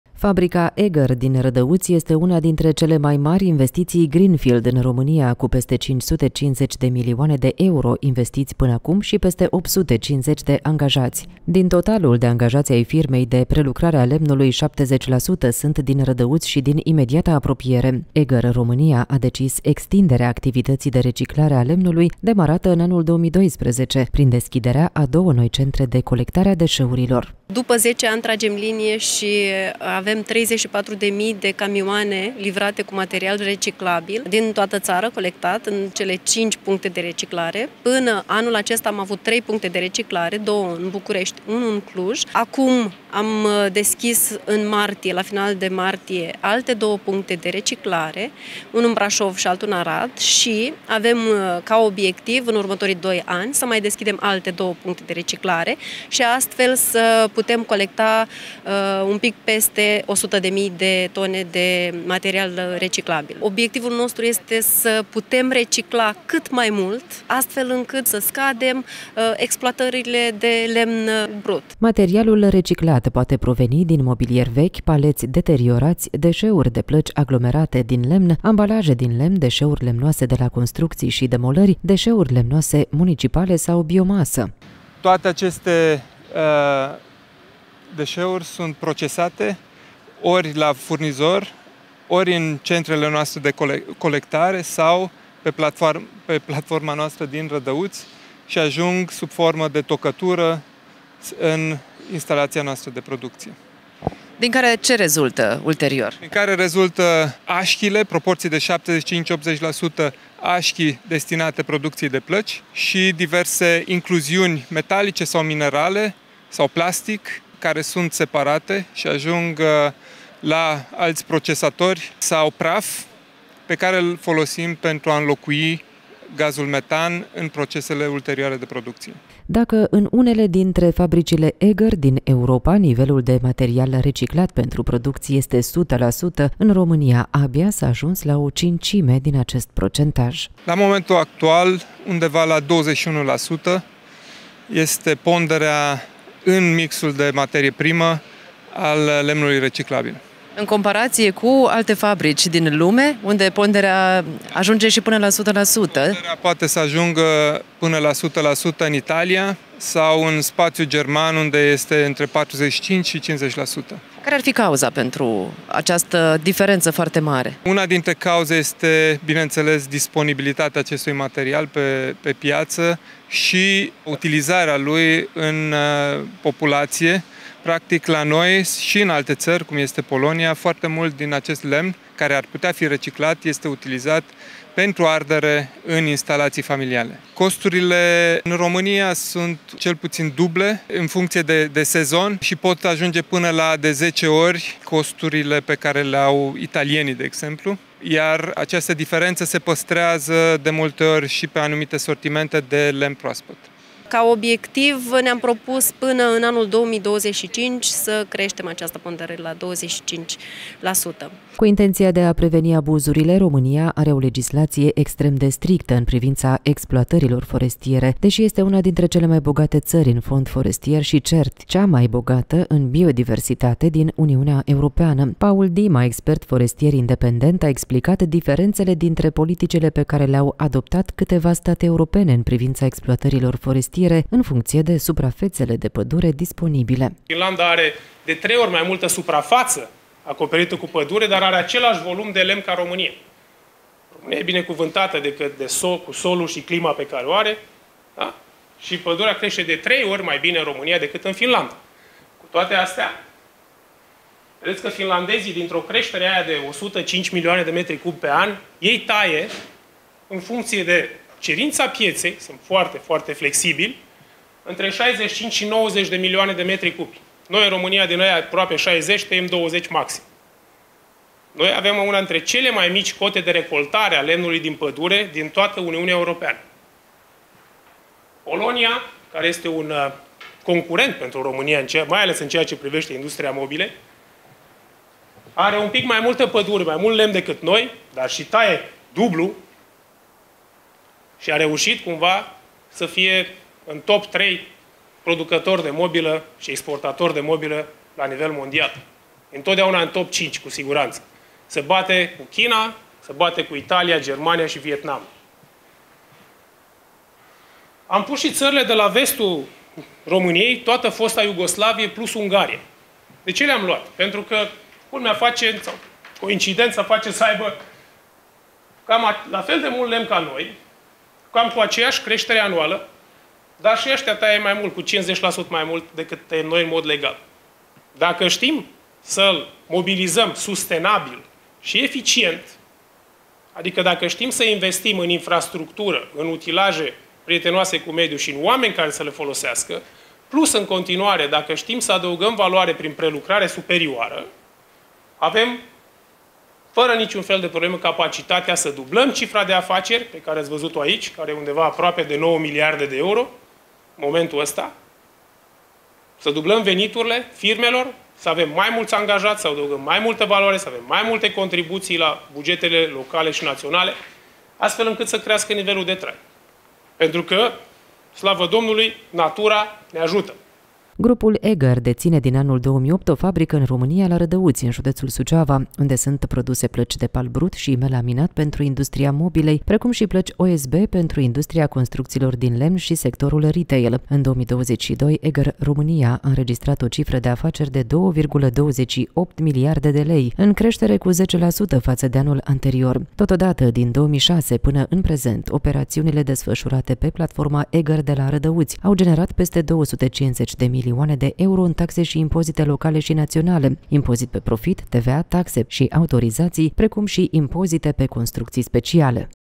EGGER Romania extinde activitatea de reciclare a lemnului
Fabrica EGGER din Rădăuţi este una dintre cele mai mari investiţii greenfield în România, cu peste 550 de milioane de euro investiți până acum şi peste 850 angajaţi. Din totalul de angajaţi ai firmei de prelucrare a lemnului, 70% sunt din Rădăuţi şi din imediata apropiere.
EGGER Romania a decis extinderea activității de reciclare a lemnului, demarată în 2012, prin deschiderea a două noi centre de colectare a deșeurilor.